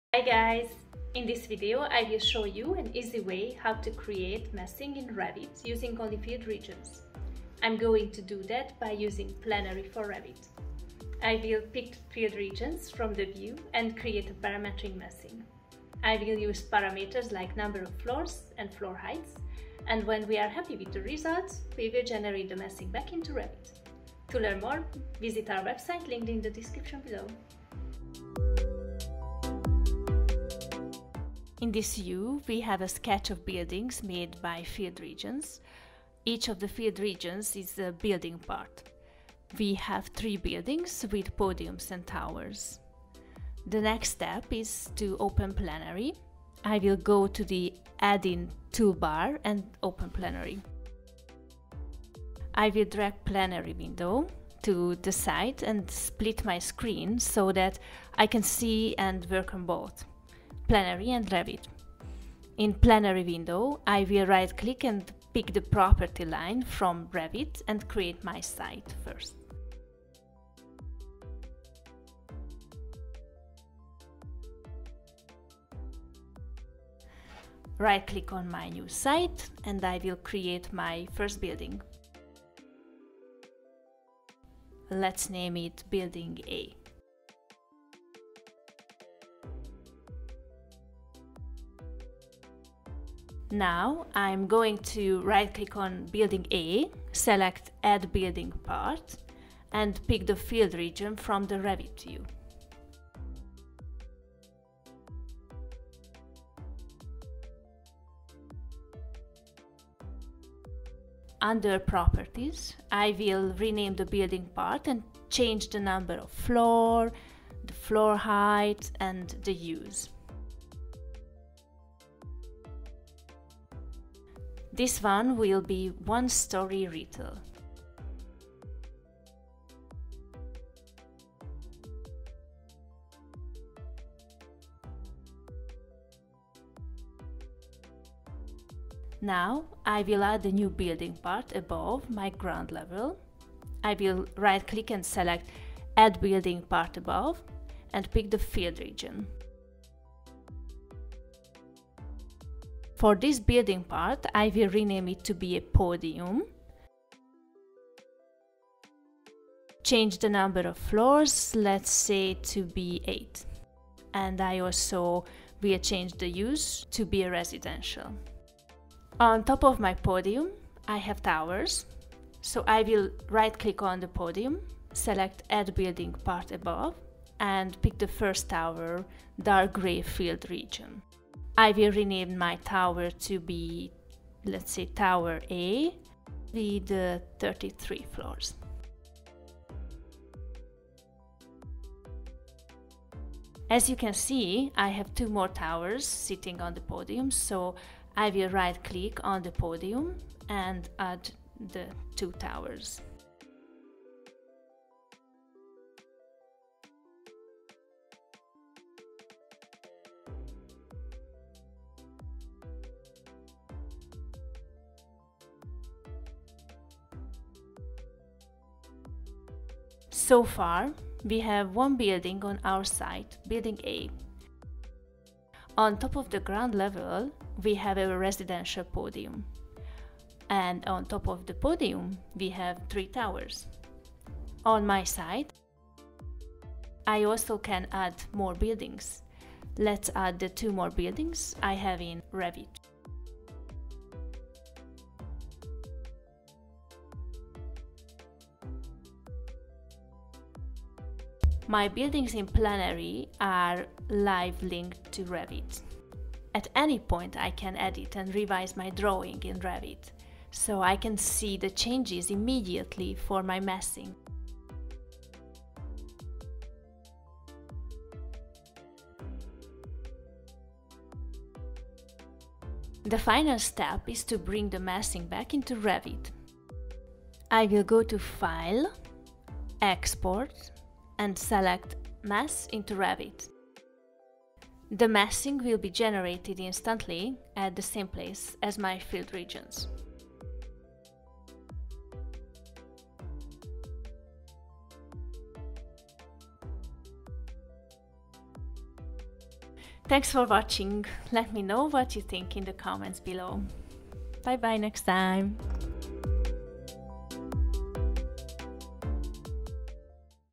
Quick Massing in Revit Using Filled Regions - Planary Add-in
This short video is on how to generate massing in Revit using the Planary add-in.
Planary is a dynamic and intuitive tool for early-stage planning
inside Autodesk revit .

0:00 Intro
0:30 Create massing using Planary
4:30 Generate massing